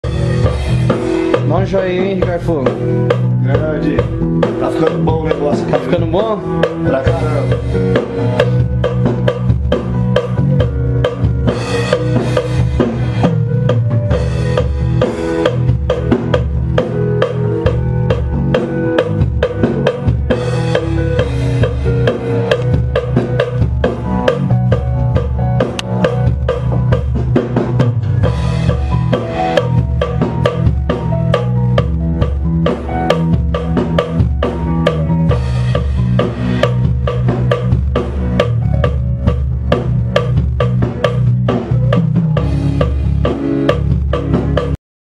ministerio shabach cd gravação da bateria studio
cd ministerio shabach gravação de bateria estudio r.a com ricardo fogo e washington fontes. marilia sp